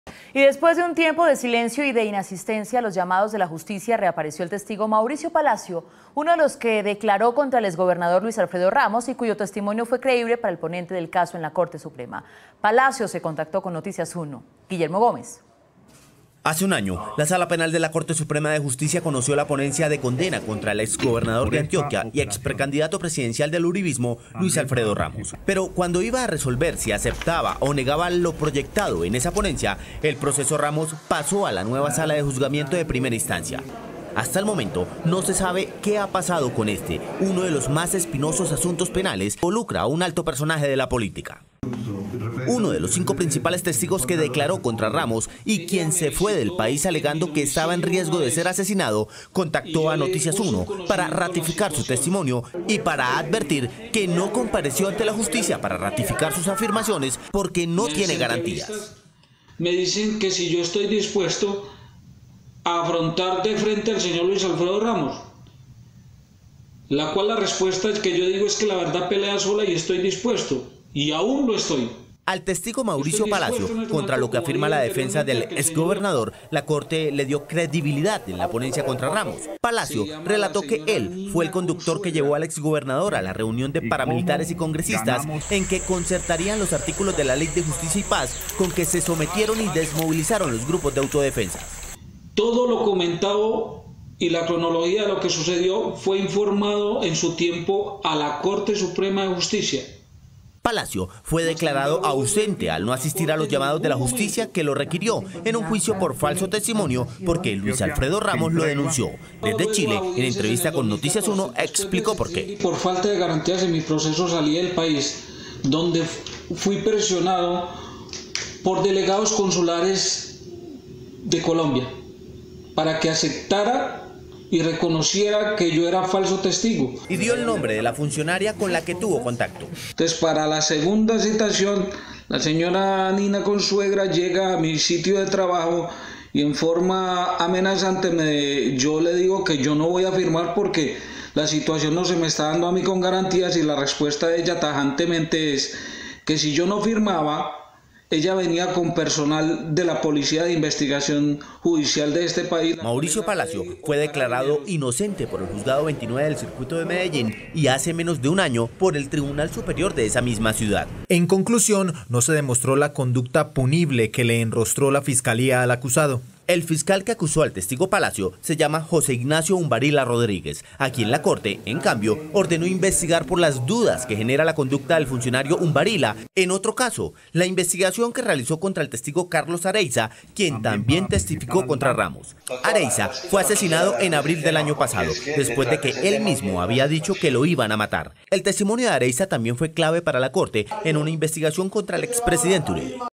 Reapareció Mauricio Palacio, testigo clave en caso contra exgobernador Luis Alfredo Ramos
Después de un tiempo de silencio y de inasistencia a los llamados de la justicia, reapareció el testigo Mauricio Palacio, uno de los que declaró contra el exgobernador Luis Alfredo Ramos y cuyo testimonio fue creíble para el ponente del caso en la Corte Suprema, Palacio se contactó con Noticias Uno.